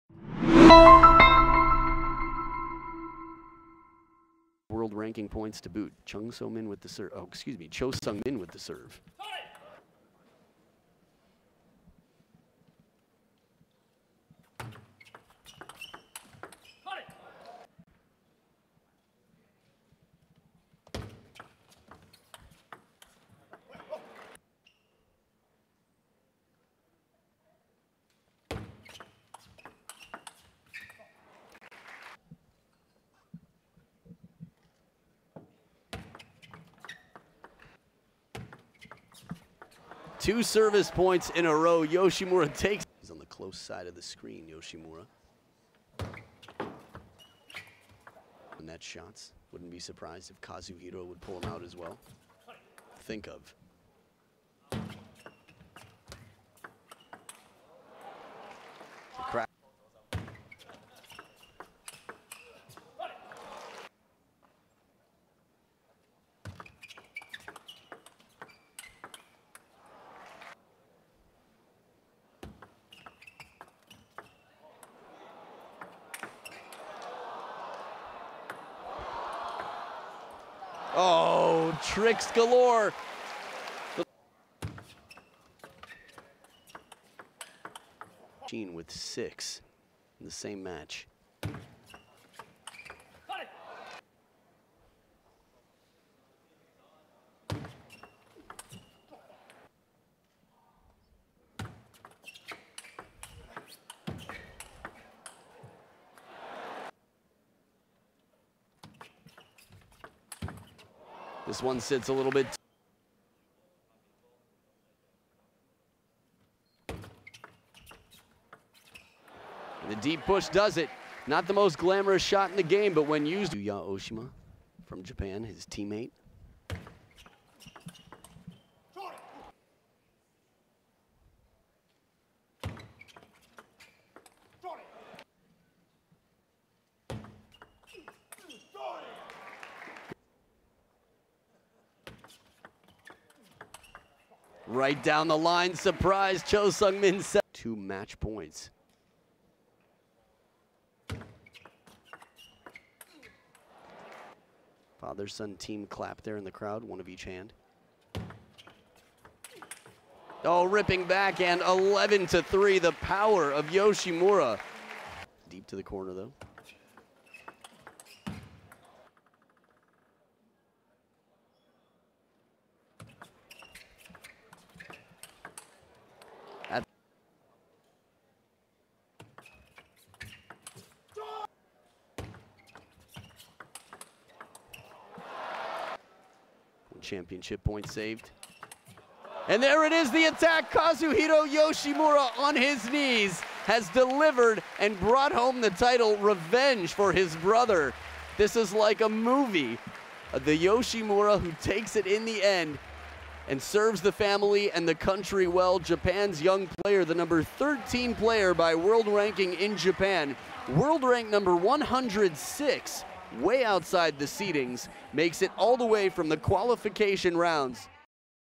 2018 Hong Kong Open Highlights | Cho Seungmin vs Kazuhiro Yoshimura (Final)
Review all the highlights from the Cho Seungmin vs Kazuhiro Yoshimura (Final) Match from the 2018 Hong Kong Open